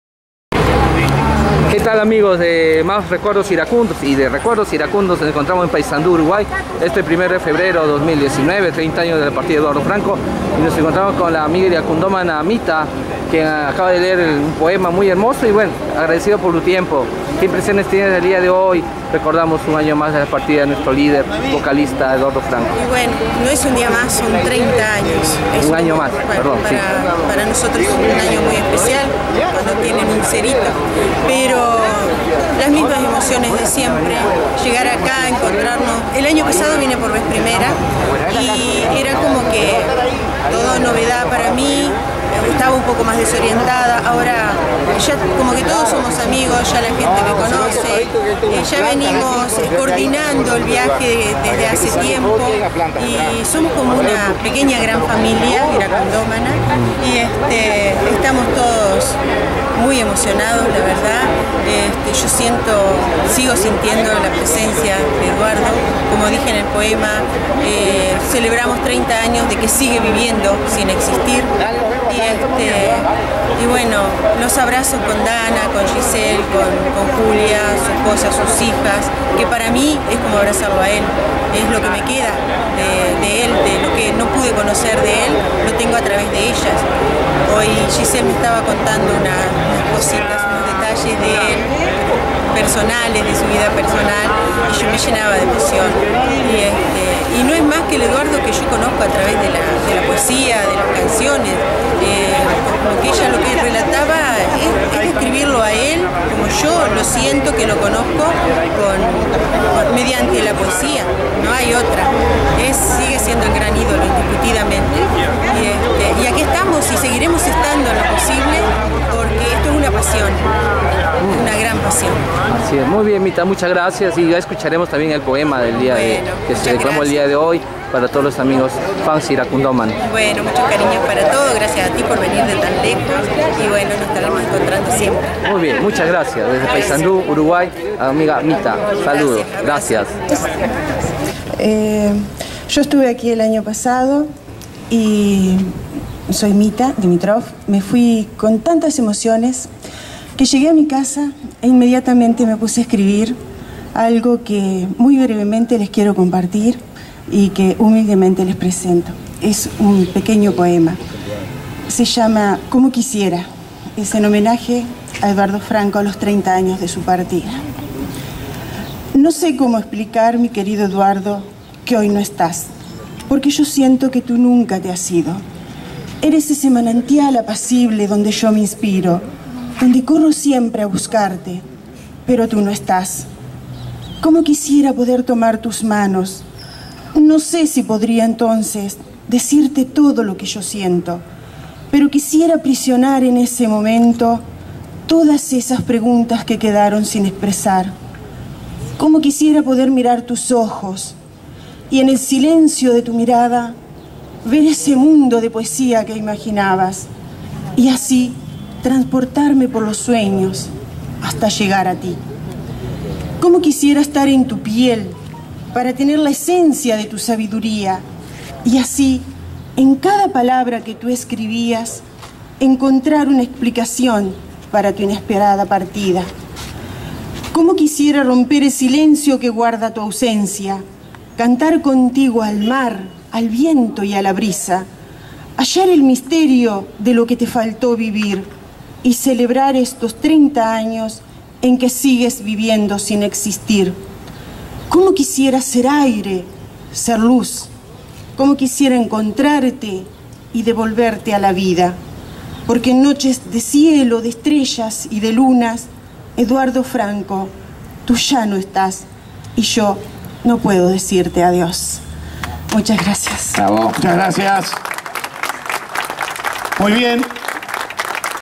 Con la fan iracundomana Mita Kalina Dimitroff Sierra desde Paysandu--Uruguay--2019
Recuerdos  del  1  de  febrero 2019   en ocasion por los 30 años  de la partida fisica del cantante   de LOS IRACUNDOS-- EDUARDO  FRANCO . Unas vivencias  por parte  de Mita y el poema  dedicado a EDUARDO  FRANCO en la  ceremonia  de  UNA  FLOR PARA EDUARDO en plena  Avenida España , en el  busto a E.  FRANCO .